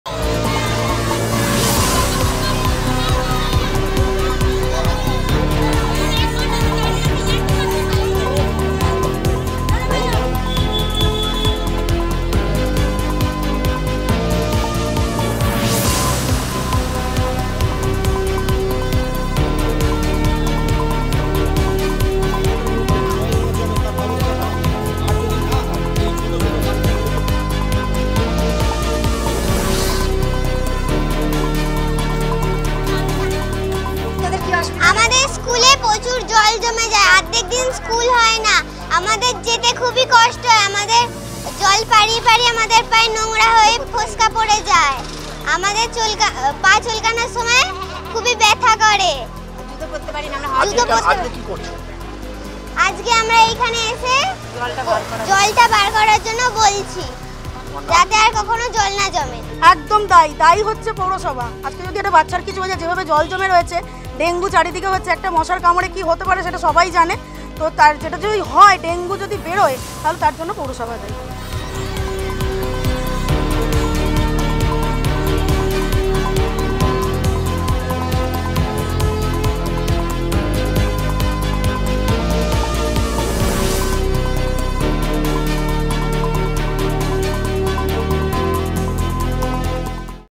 School waterlogged, students stage protest in front of Dankuni municipality । Sangbad Pratidin
School waterlogged, students stage protest in front of Dankuni municipality.